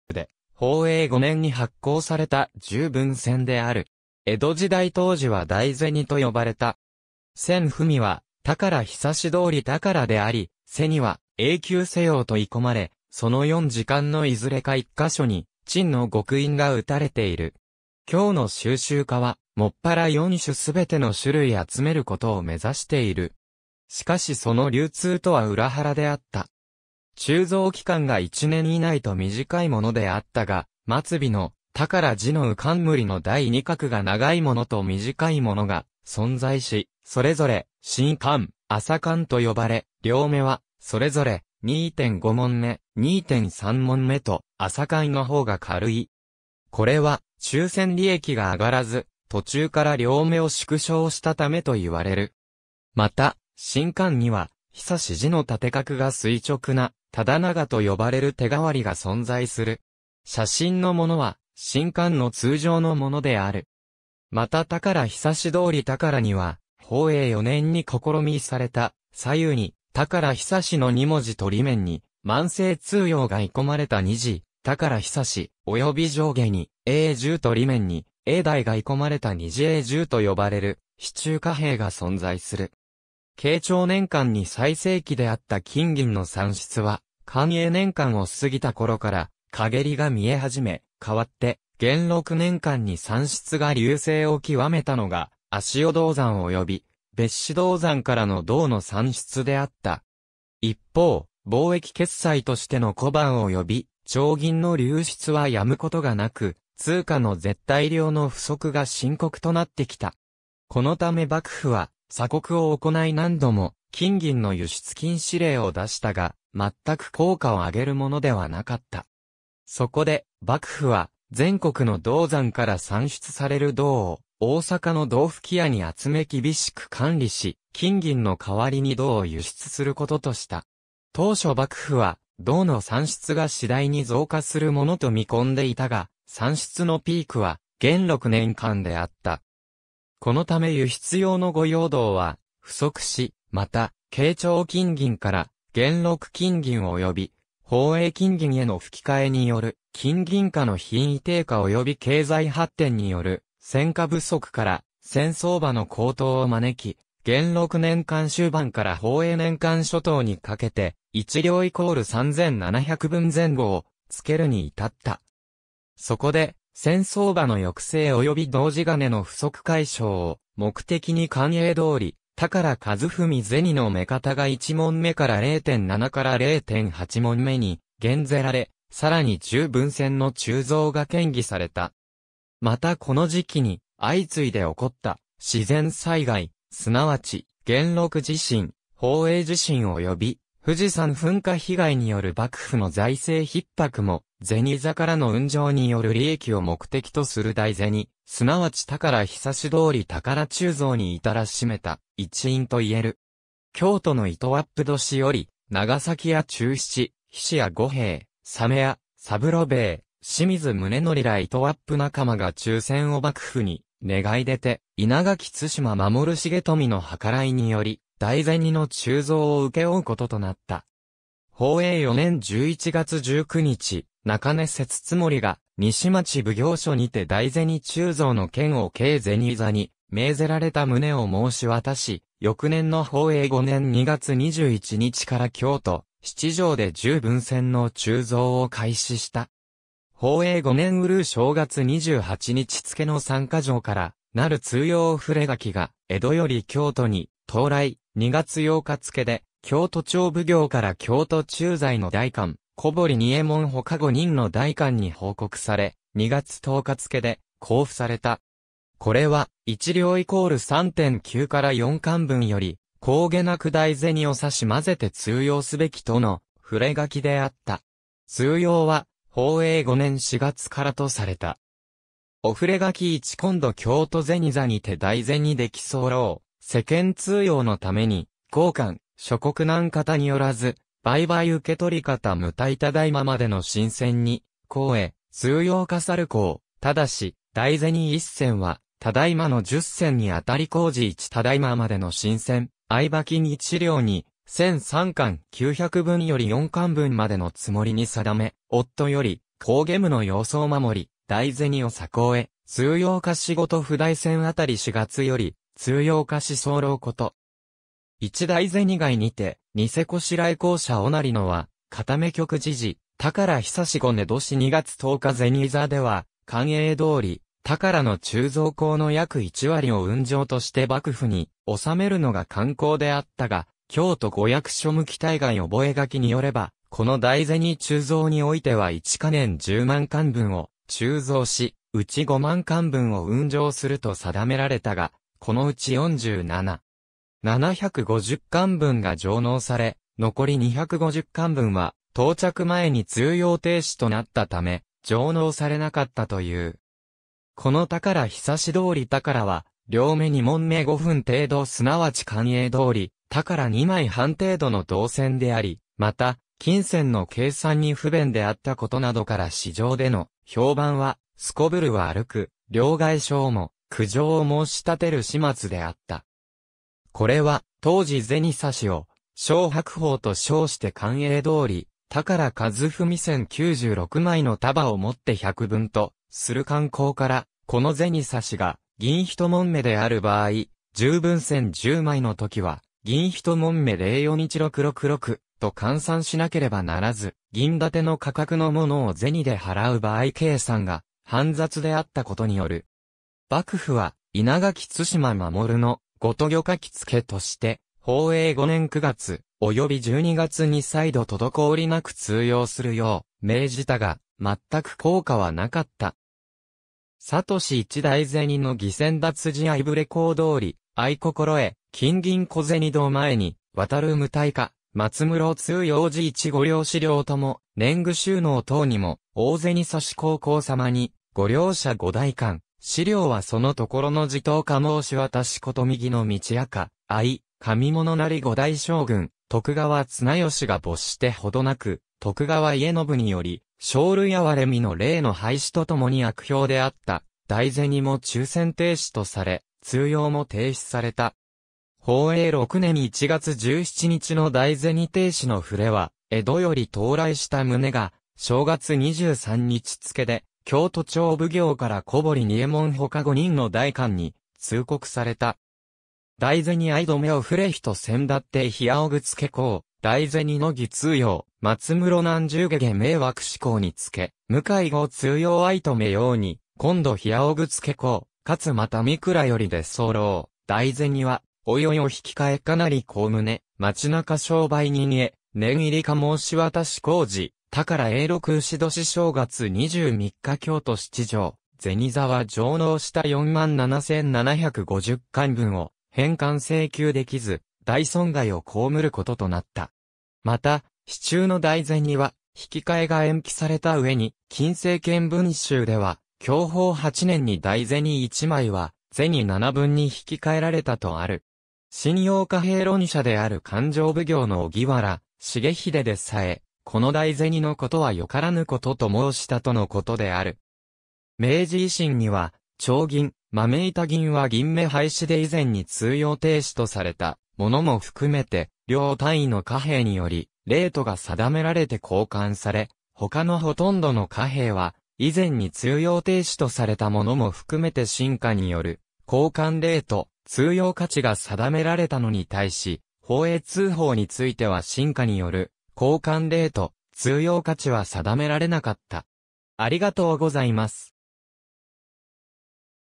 宝永通宝
寳永通寳 宝永通宝（ほうえいつうほう）とは、江戸時代に鋳造された銭貨の一種で宝永5年（1708年）に発行された十文銭である。
江戸時代当時は大銭（おおぜに）と呼ばれた。
銭文は「寳永通寳」であり、背（裏面）には「永久世用」と鋳込まれ、その四字間のいずれか1箇所に「珍」の極印が打たれている。
今日の蒐集家は専ら4種全ての種類集めることを目指している。
しかしその流通とは裏腹であった。
鋳造期間が一年以内と短いものであったが、末尾の「寳」字のウ冠の第二画が長いものと短いものが存在し、それぞれ「深冠」、「浅冠」と呼ばれ、量目はそれぞれ、2.5匁（9.37グラム）、2.3匁（8.62グラム）と「浅冠」の方が軽い。
これは鋳銭利益が上がらず、途中から量目を縮小したためと言われる。
また「深冠」には「永」字の縦画が垂直な「直永」と呼ばれる手代わりが存在する。
写真のものは「深冠」の通常のものである。
また寳永通寳には、宝永4年（1707年）に試鋳された、左右に「寳永」の二文字と裏面に「万世通用」が鋳込まれた二字寳永（にじほうえい）、および上下に「永十」と裏面に「永大」が鋳込まれた二字永十（にじえいじゅう）と呼ばれる試鋳貨幣が存在する。
慶長年間に最盛期であった金銀の産出は寛永年間を過ぎた頃から陰りが見え始め、代わって元禄年間に産出が隆盛を極めたのが足尾銅山および別子銅山からの銅の産出であった。
一方、貿易決済としての小判および丁銀の流出は止むことが無く、通貨の絶対量の不足が深刻となってきた。
このため幕府は、鎖国を行い何度も金銀の輸出禁止令を出したが、全く効果をあげるものではなかった。
そこで幕府は全国の銅山から産出される銅を、大坂の銅吹屋に集め厳しく管理し、金銀の換わりに銅を輸出することとした。
当初幕府は銅の産出が次第に増加するものと見込んでいたが、産出のピークは元禄年間であった。
このため輸出用の御用銅は不足し、また慶長金銀から元禄金銀および宝永金銀への吹き替えによる金銀貨の品位低下および経済発展による銭貨不足から銭相場の高騰を招き、元禄年間終盤から宝永年間初頭にかけて、一両＝3,700文前後をつけるに至った。
そこで銭相場の抑制および銅地金の不足解消を目的に寛永通寳一文銭の目方が1匁から0.7-0.8匁に減ぜられ、さらに十文銭の鋳造が建議された。
またこの時期に相次いで起こった自然災害、すなわち元禄地震、宝永地震および富士山噴火被害による幕府の財政逼迫も、銭座からの運上による利益を目的とする大銭すなわち寳永通寳鋳造に至らしめた一因といえる（『折たく柴の記』）。
京都の糸割符年寄り、長崎屋忠七・菱屋五兵衛・鮫屋三郎兵衛・清水宗仙ら糸割符仲間が鋳銭を幕府に願い出て、稲垣対馬守重富の計らいにより、大銭の鋳造を請け負うこととなった。
宝永4年11月19日（1707年12月12日）、中根摂津守が西町奉行所にて大銭鋳造の件を京銭座に命ぜられた旨を申し渡し、翌年の宝永5年2月21日（1708年4月11日）から京都七条で十文銭の鋳造を開始した。
宝永5年閏正月28日（1708年3月20日）付の三ヶ条から成る通用触書が江戸より京都に到来、2月8日（1708年3月29日）付で京都町奉行から京都駐在の代官小堀仁右衛門他5人の代官に報告され、2月10日（1708年3月31日）付で公布された。
これは「一両＝3.9～4貫文より高下なく大銭を差混ぜて通用すべき」との触書であった。
通用は宝永5年4月からとされた。
　　　御触書 一　今度京都銭座にて大銭出来候、世間通用の為に候間、諸国何方によらず、売買請取方無滞只今迄の新銭に交へ、通用可申候、但大銭一銭は、只今の十銭に当り候事 一　只今迄の新銭、相庭金壱両に銭三貫九百文より四貫文迄の積りに定め、夫より高下無之様相守り、大銭を差交へ通用可仕事 　　　附大銭当四月より通用可仕候事
一　大銭外にて、似せ拵候者於有之は、可為曲事事 　　　寳永五子年二月十日 銭座では寛永通寳の鋳造高の約一割を運上として幕府に納めるのが慣行であったが、『京都御役所向大概覚書』によれば、この大銭鋳造においては一カ年十万貫文を鋳造し、うち五万貫文を運上すると定められたが、この内47,
750貫文が上納され、残り250貫文は到着前に通用停止となったため上納されなかったという。
この寳永通寳は量目2匁5分程度すなわち寛永通寳2枚半程度の銅銭であり、また金銭の計算に不便であったことなどから市場での評判はすこぶる悪く、両替商も苦情を申し立てる始末であった。
これは当時銭緡（ぜ...